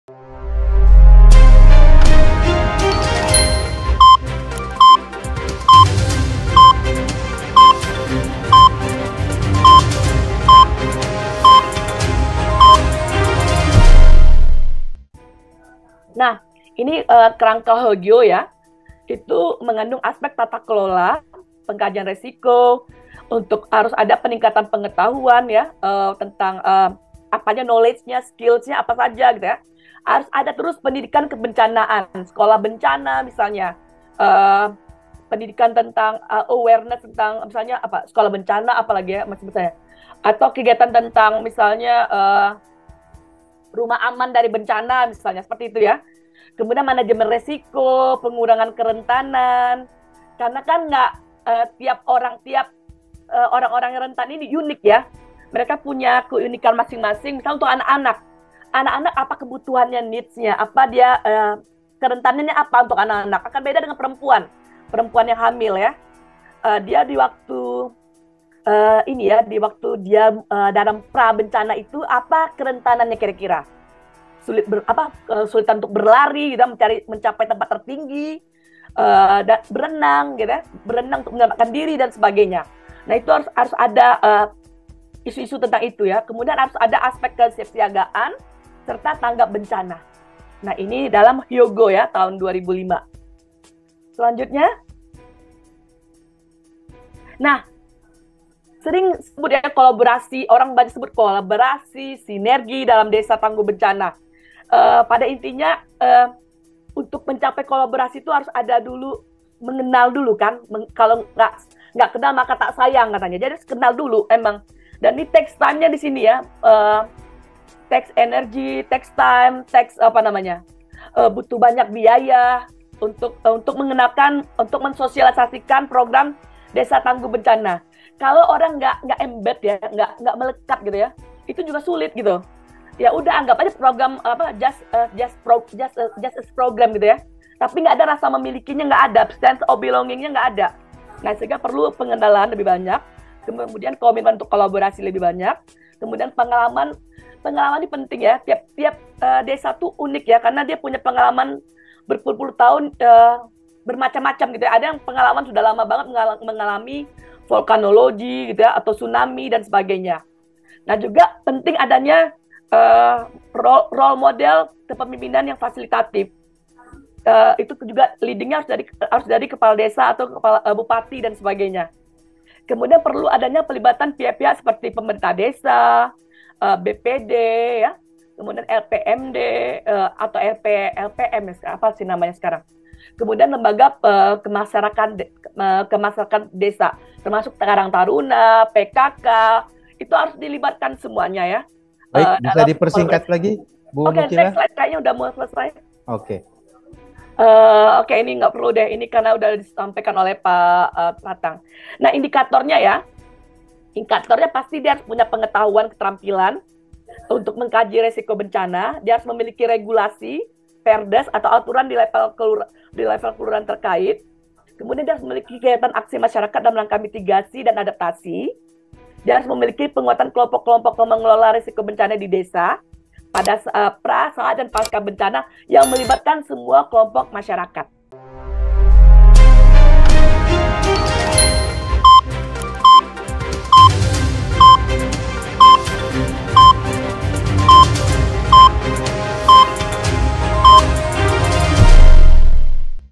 Kerangka Aksi Hyogo Dalam Menanggulangi Bencana
Kerangka Hyogo merupakan kerangka kerja yang diberlakukan pada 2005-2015 sebagai panduan global yang digunakan oleh berbagai negara-negara di dunia, dalam upaya pengurangan risiko bencana. Upaya pengurangan risiko bencana secara global telah dilakukan melalui beberapa kesepakatan antar berbagai negara. Salah satu kesepakatan yang mendasari upaya pengurangan risiko bencana secara global adalah Kerangka Aksi Hyogo yang diberlakukan pada tahun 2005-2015 silam dan ditandatangani oleh 168 negara.

Kerabat Pemirsa TV Desa, apa pendapat kerabat pada tayangan TV Desa ?
Tulis di kolom komentar dengan cara ( Nama - Kabupaten/Kota -  Pendapat Kerabat)